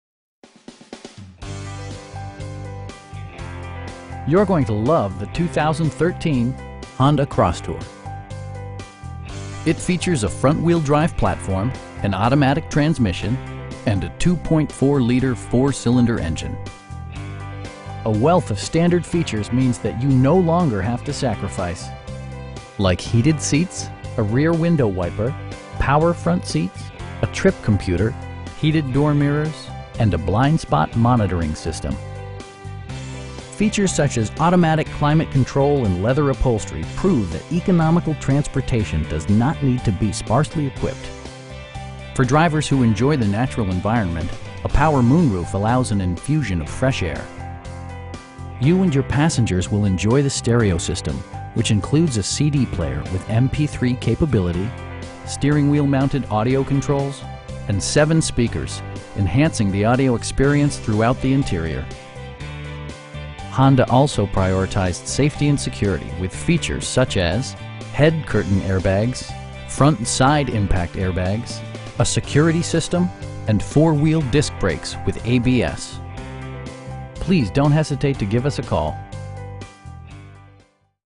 2013 Honda Crosstour EX-L FWD
You're going to love the 2013 Honda Crosstour. It features a front-wheel-drive platform, an automatic transmission, and a 2.4 liter 4 cylinder engine. A wealth of standard features means that you no longer have to sacrifice. Like heated seats, delay-off headlights, a rear window wiper, power front seats, a trip computer, heated door mirrors, power windows, and a blind spot monitoring system. Features such as automatic climate control and leather upholstery prove that economical transportation does not need to be sparsely equipped. For drivers who enjoy the natural environment, a power moon roof allows an infusion of fresh air. You and your passengers will enjoy the stereo system, which includes a CD player with MP3 capability, steering wheel mounted audio controls, and 7 speakers, providing excellent sound throughout the cabin. Honda also prioritized safety and security with features such as: head curtain airbags, front SIDE impact airbags, traction control, brake assist, anti-whiplash front head restraints, a security system, and 4 wheel disc brakes with ABS. Electronic stability control ensures solid grip atop the road surface, no matter how challenging the driving conditions. We know that you have high expectations, and we enjoy the challenge of meeting and exceeding them! Please don't hesitate to give us a call.